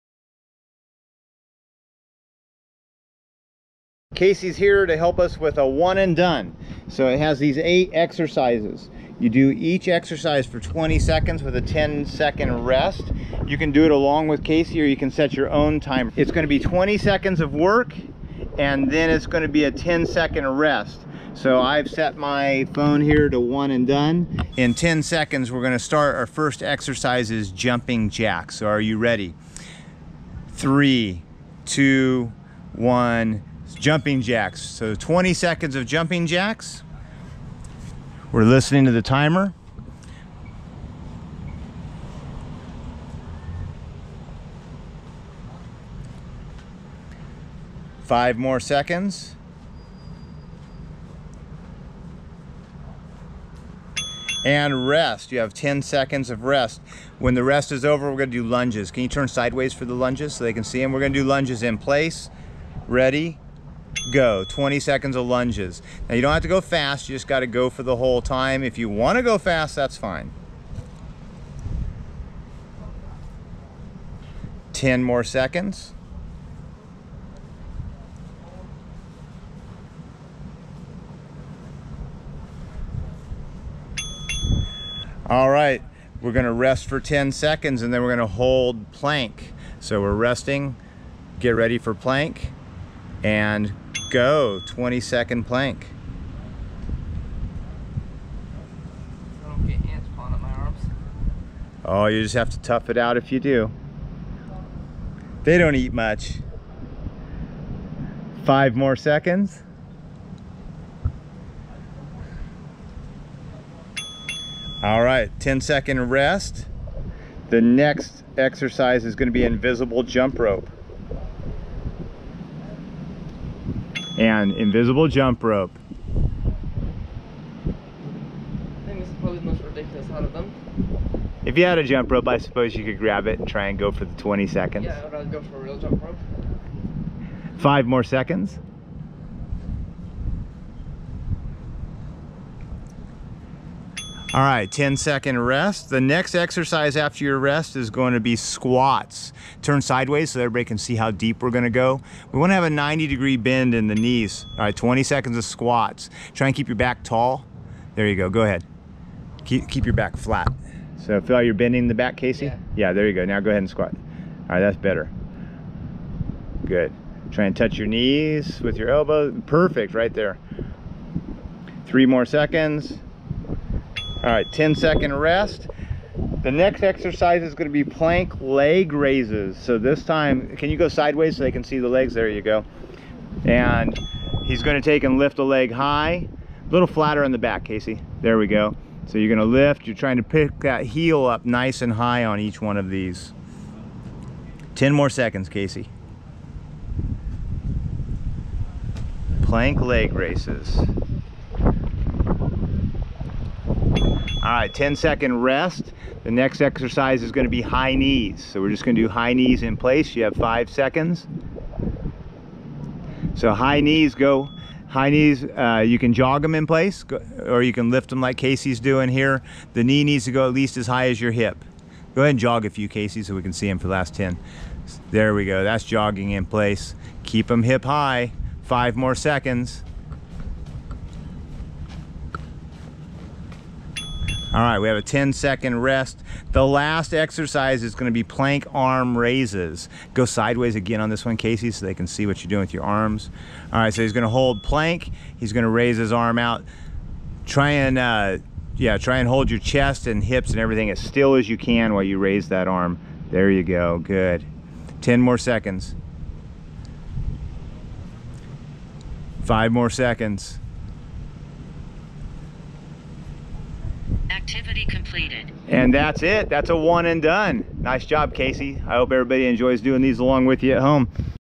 One and Done
Follow along video for the DCS Pe Lesson "One and Done" 6th-8th grade